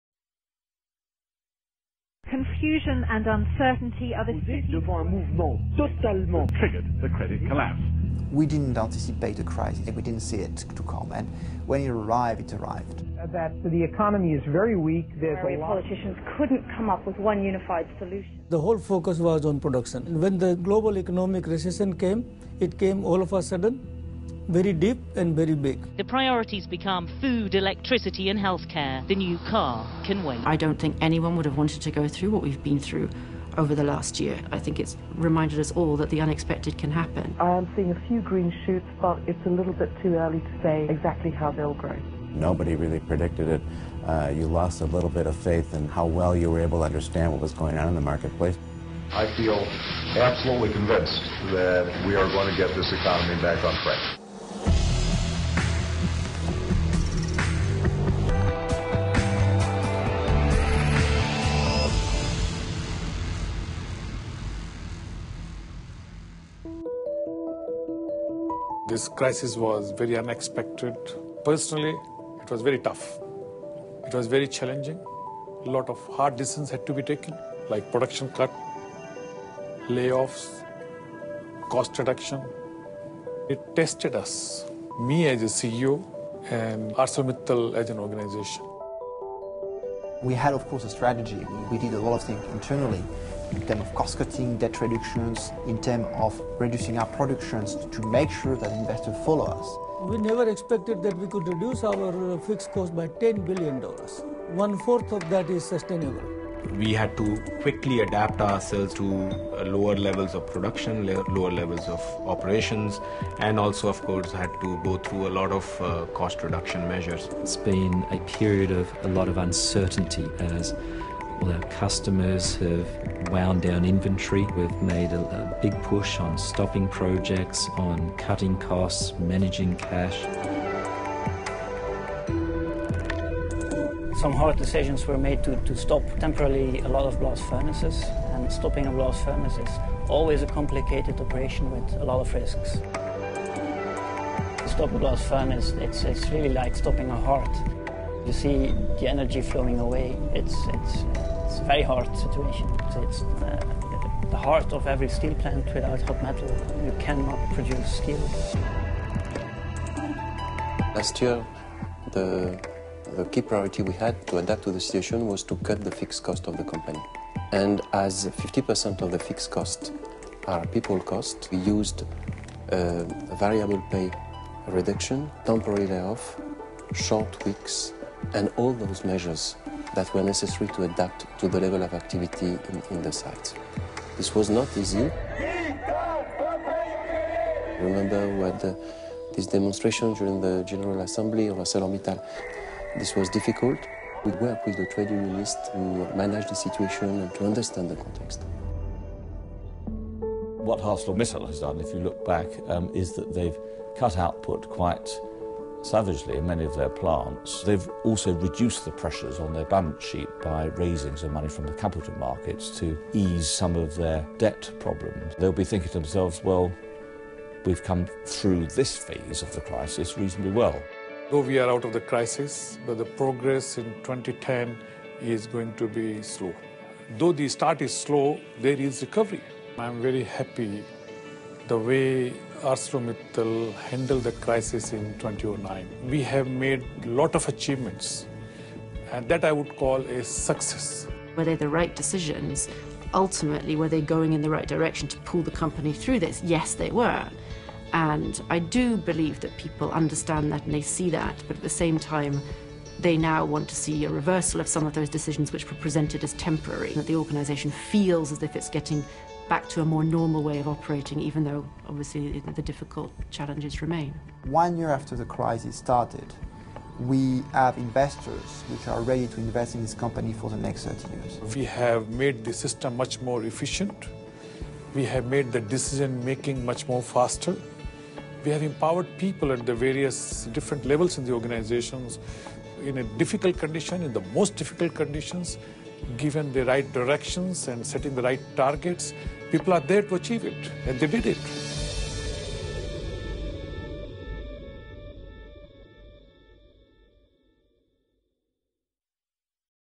ArcelorMittal Web TV 2010 - Episode 1 - Emerging from the crisis
Web TV 2010 kicks off with an episode encapsulating the challenging environment of the last 18 months. The episode points a camera at the repercussions of the crisis, the actions taken by ArcelorMittal and the sentiments as we look at the road to recovery.